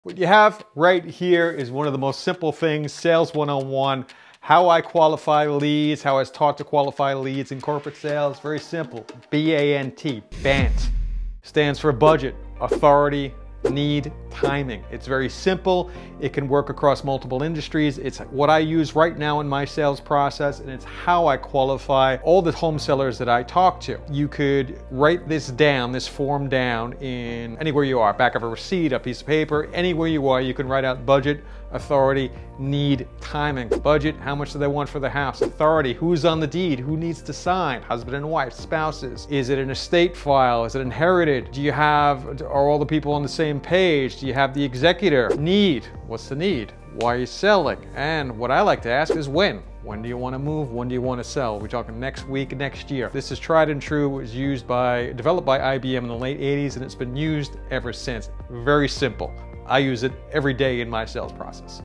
How To Qualify Motivated Seller Leads
What is the best way to qualify motivated seller leads?  There are many companies selling secret questions, tactics, tricky closes, etc.  I use none of it.  In my sales process, I use the B.A.N.T. formula which stands for Budget, Authority, Need, and Timing.

Talking to motivated home sellers is like talking to any other human being.  Conversations don't follow a rigid logical structure.  The B.A.N.T framework allows real estate investors to be flexible and conversational when qualifying motivated sellers.

Watch this video to learn more on how I qualify motivated home sellers.